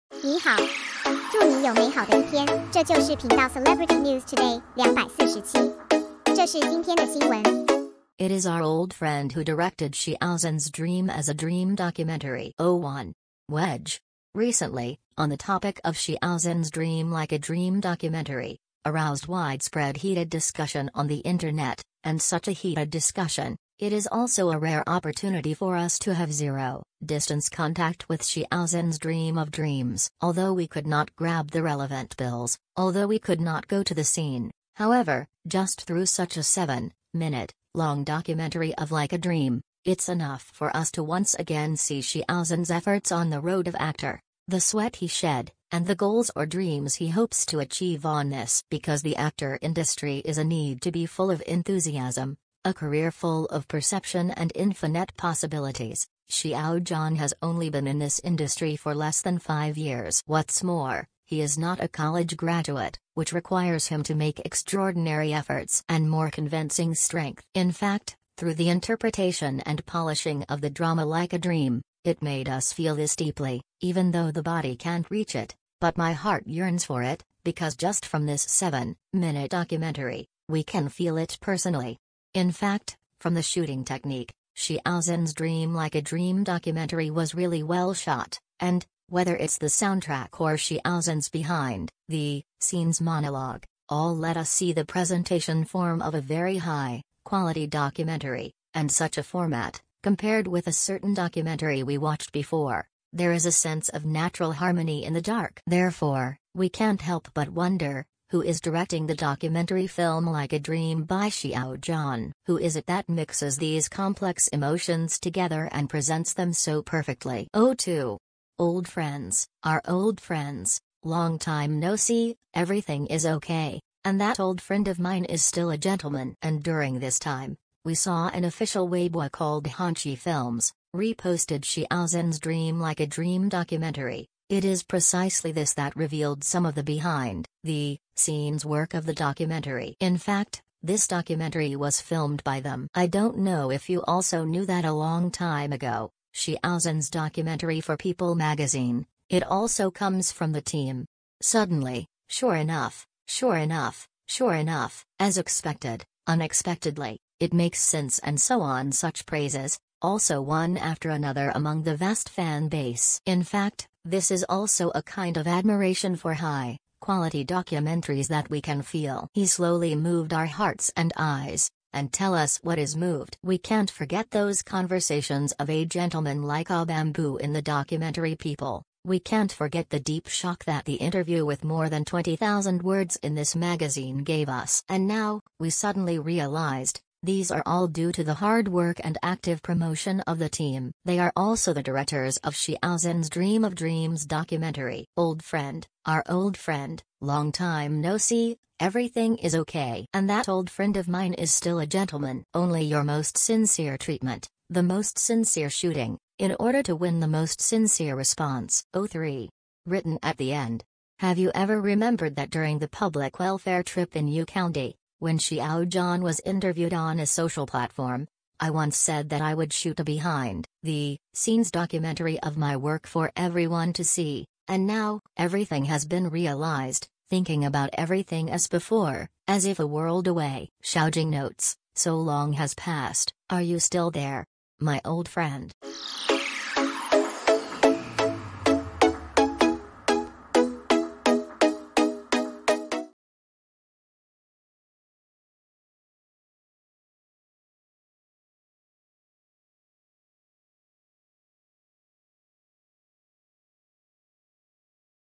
It is our old friend who directed Xiao Zhan's "Dream as a Dream" documentary.
Celebrity News Today 247 2021/09/28 : It is our old friend who directed Xiao Zhan's "Dream as a Dream" documentary.
 It is our old friend who directed Xiao Zhan's "Dream as a Dream" documentary.  01. Wedge. Recently, on the topic of Xiao Zhan’s "Dream Like a Dream" documentary,  Aroused widespread heated discussion on the Internet, and such a heated discussion,  It is also a rare opportunity for us to have zero-distance contact with Xiao Zhan’s "Dream of Dreams".  Althoug